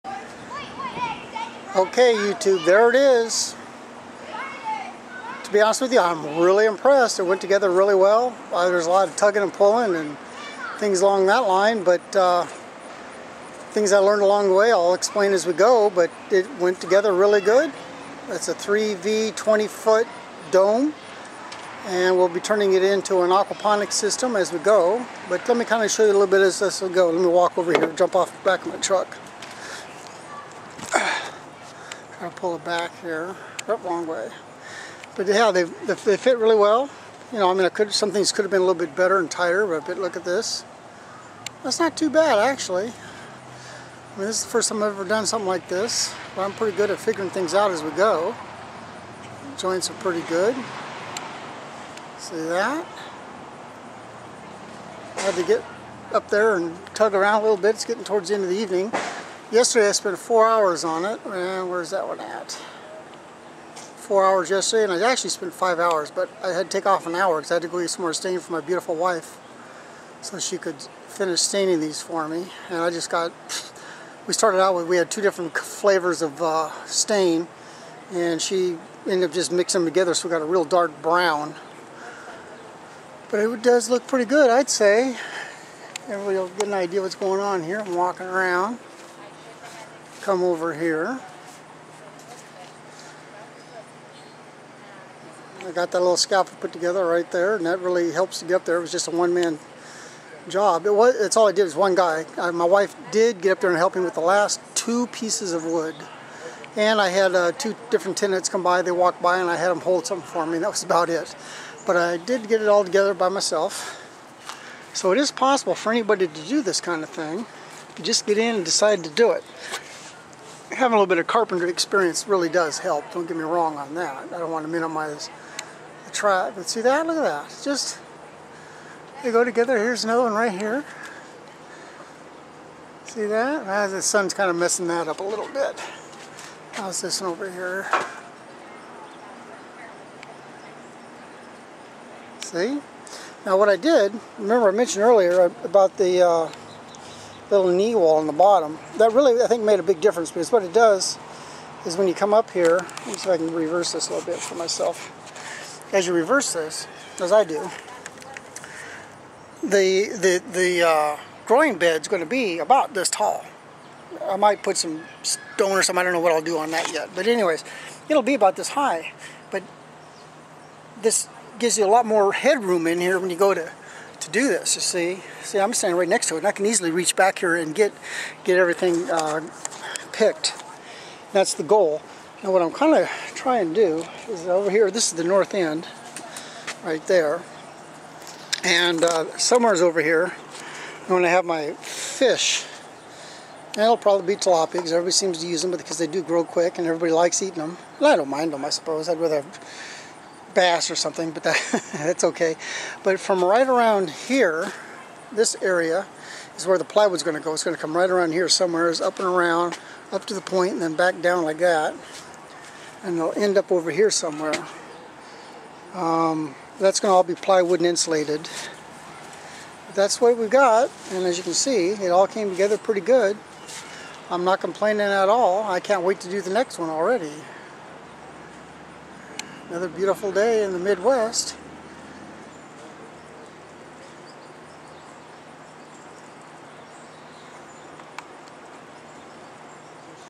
video #9 the completed dome framing!
brief description of the finished framed no spoke geodesic dome green house. Encouragement for DIYer's.